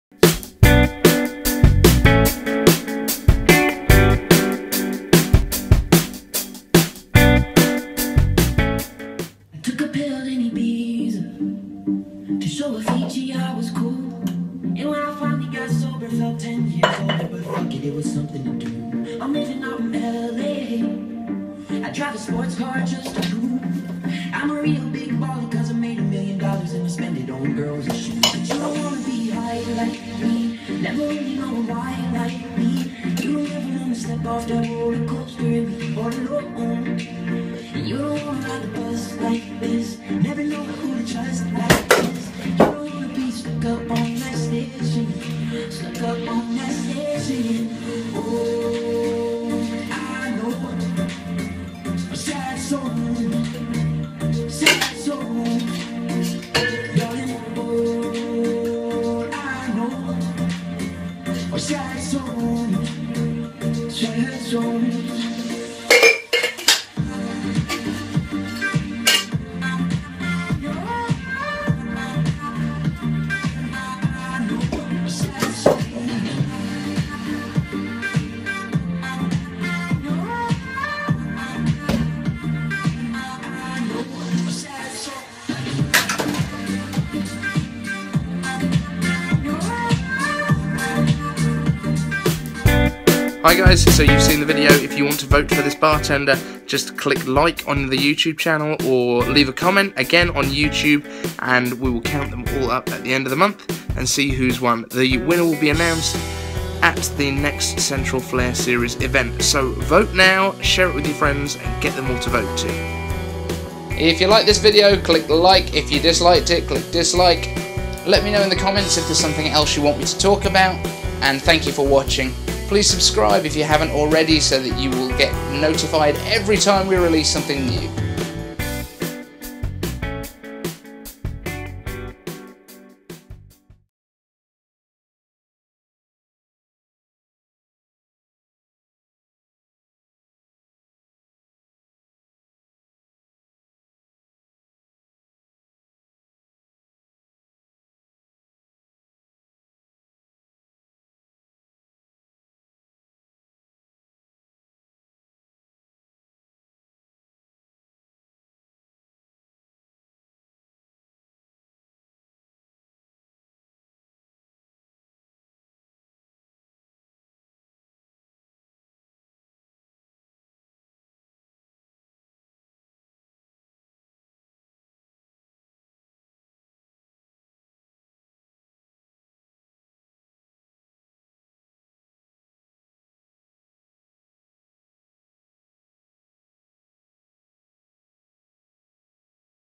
BartenderHQ: Tomáš Bielčík Online Flair Competition Entry
Flair Bartender Tomáš Bielčík Competing in the Online Flair Bartending Competition for the Central Flair Series and BartenderHQ.com